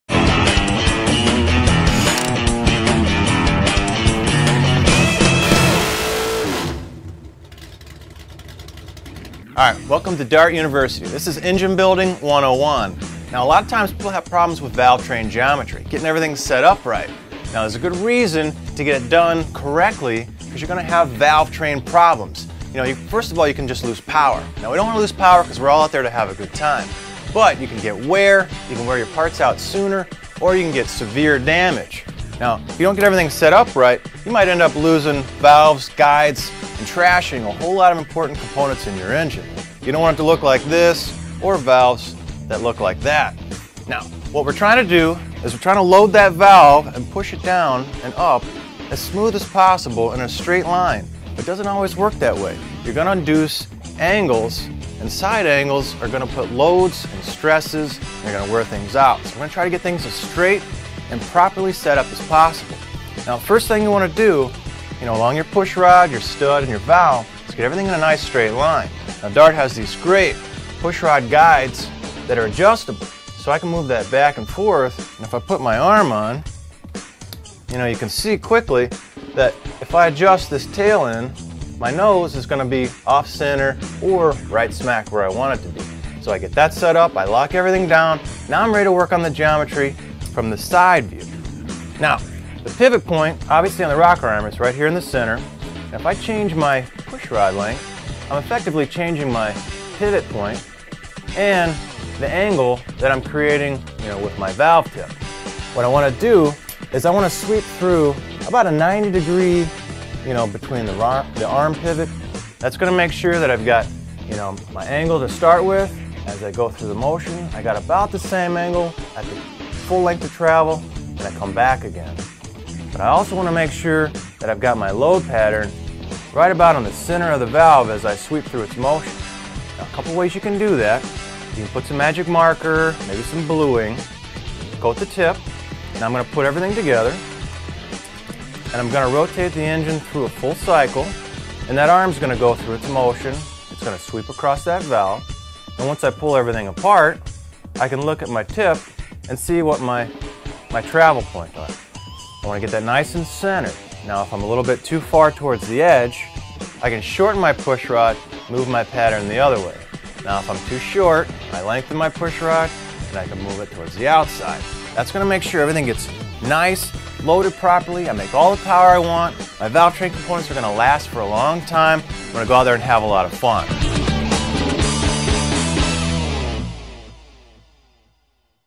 StreetLegalTV.com - Dart University, Valvetrain Geometry
Kevin Byrd from Two Guys Garage explains valvetrain geometry.